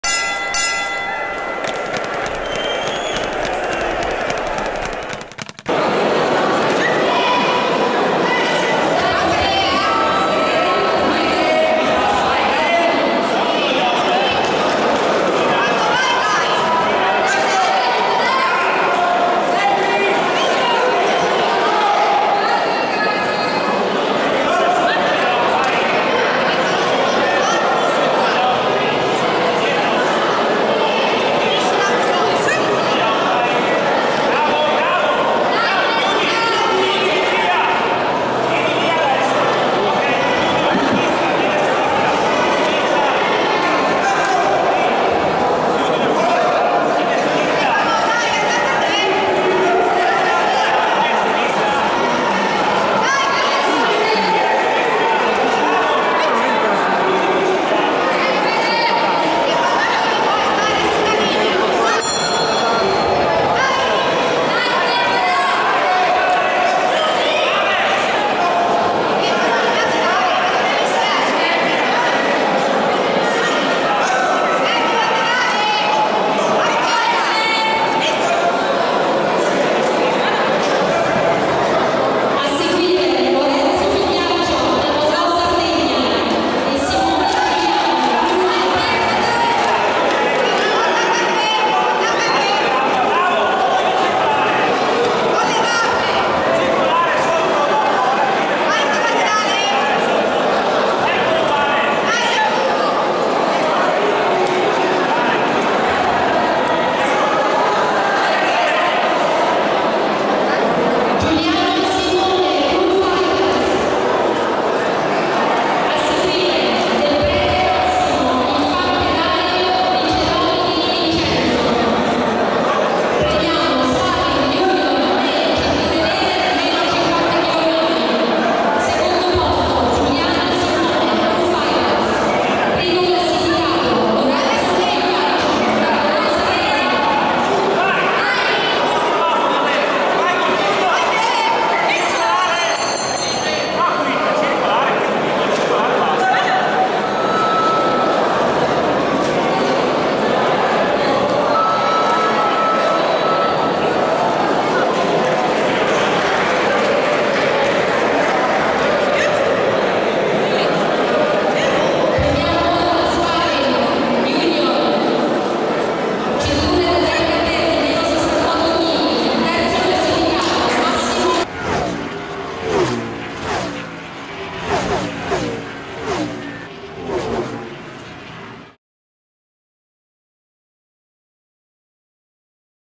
Citi Stefano Enzo vs Ceola Christopher //Campionato Taekwondo ITF NI-KKO OPEN Roma
Campionato Taekwondo ITF NI-KKO OPEN Roma presso il palatorrino. Campionato di forme e Combattimenti del 22/23 febbraio 2020.